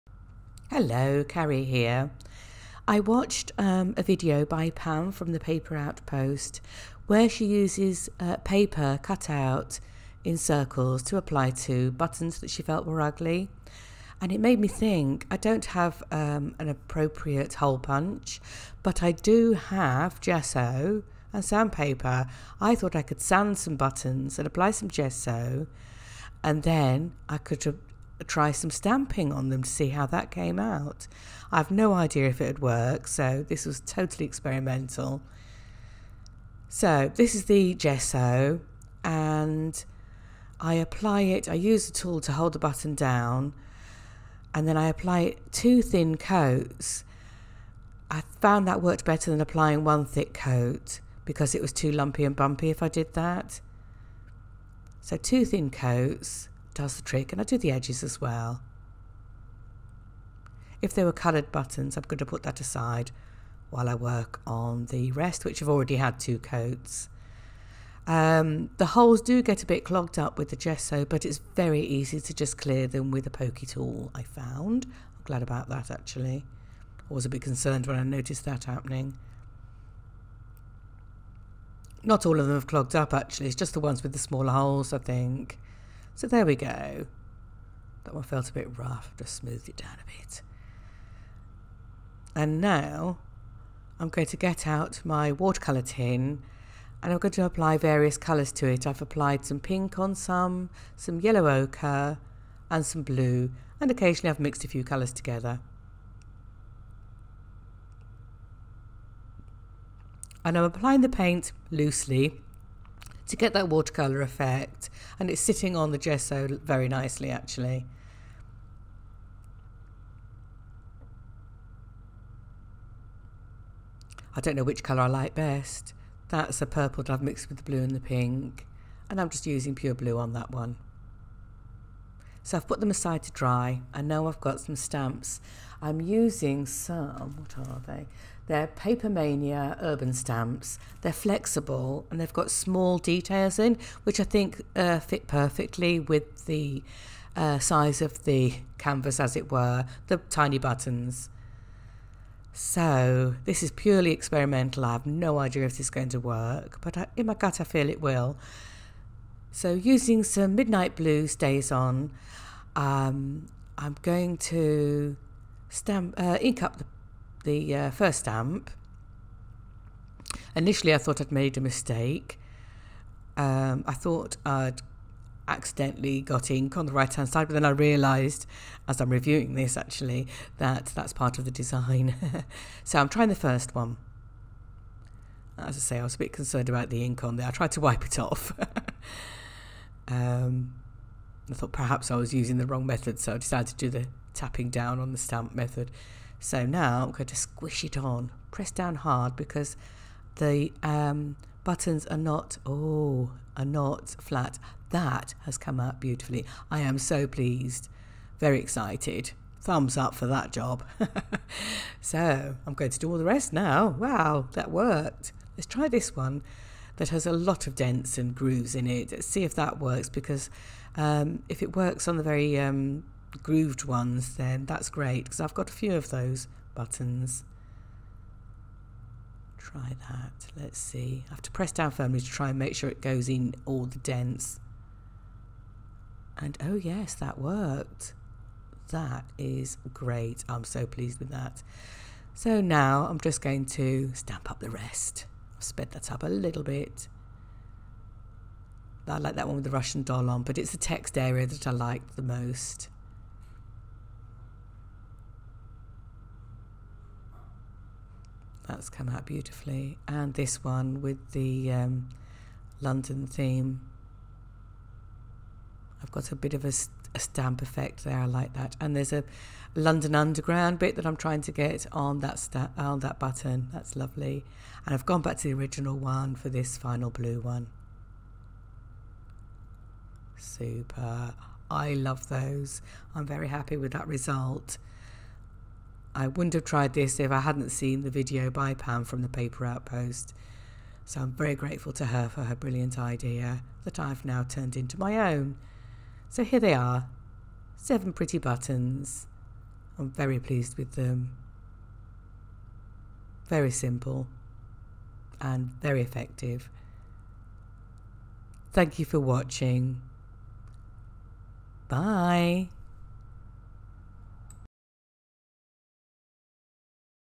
Altered Buttons For Art Journals, ATCs, Tags, etc
Turning rather boring buttons into mixed media wonders using paint and rubber stamps.